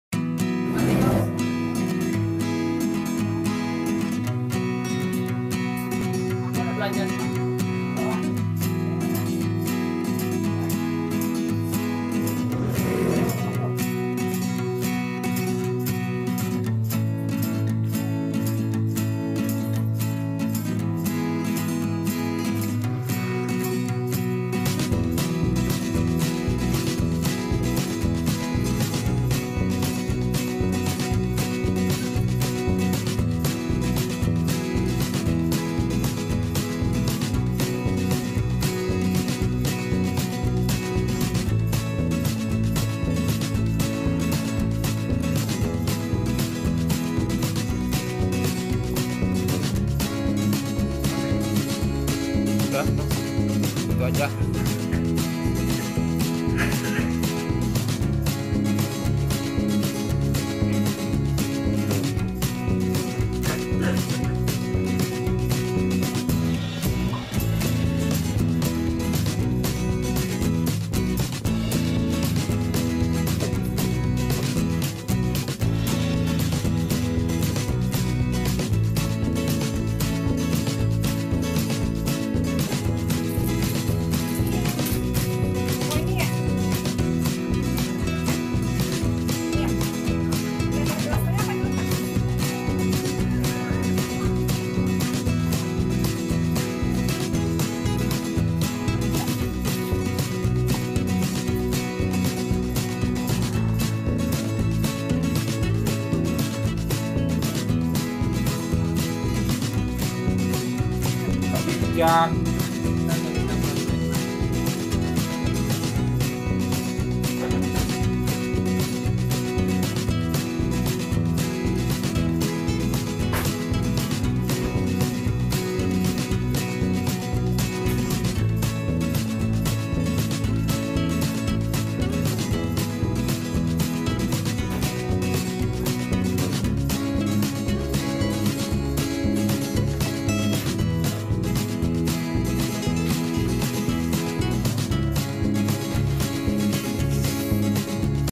DISTRIBUTOR BAHAN SPREI BISA BELI METERAN
YESS asalamualaiqum wrb video kali ini saya berbelanja bahan sprei merk star dan toko ini bisa di katakan toko bahan sprei terbesar di Tangerang
toko ini jual bahan gulungan/roll dan meteran juga alias eceran.oiya kalo ada yg mau kepo toko bahan sprei yg lengkap dan grosir silahkan comment di bawah untuk grebek toko sprei yg kalian ingin kan sampe kita langsung ngobrol ngobrol sama owner nya langsung comment yg banyak dan like share  ok👌👌 semoga video ini  bermanfaat
jangan bosan bosan untuk terus
support channel tutorial ini
                     DENGAN

untuk pemesanan material dan produk jadi

bisa DM saya secara langsung